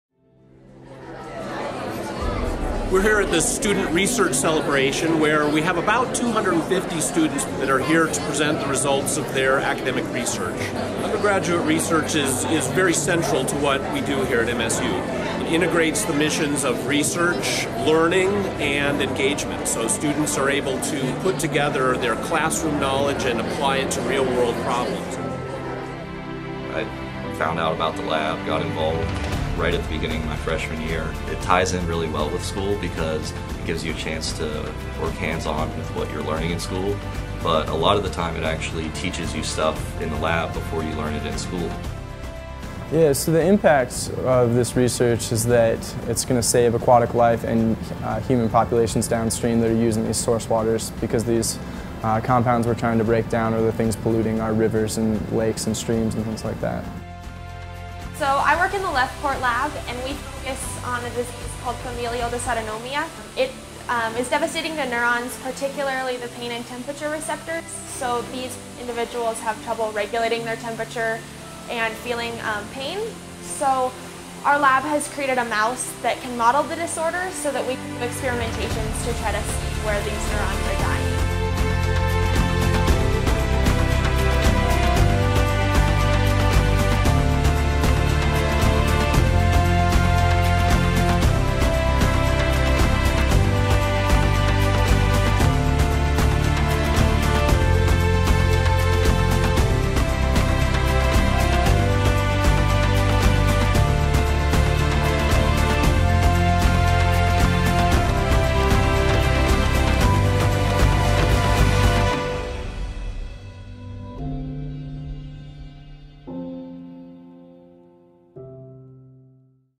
Undergraduate Research at MSU
Learn about some of the many opportunities undergraduates have to conduct research at Montana State University, one of America's top research universities.